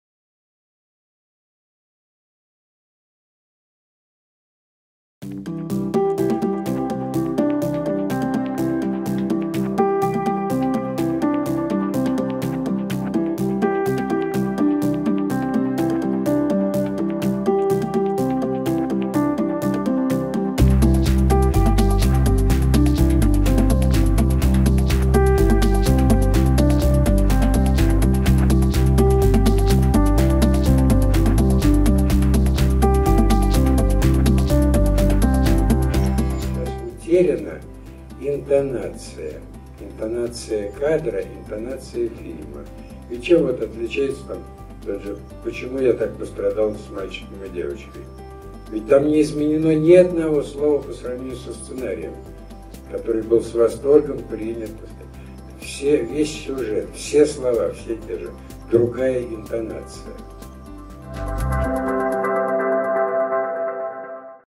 TarkovskyFest2017 :: Ретроспектива Юлия Файта
ТАРКОВСКИЙ.КОНТЕКСТ
Ретроспектива Юлия Файта XI Международный кинофестиваль им. Андрея Тарковского «Зеркало»

XI Andrey Tarkovsky International Film Festival “Zerkalo”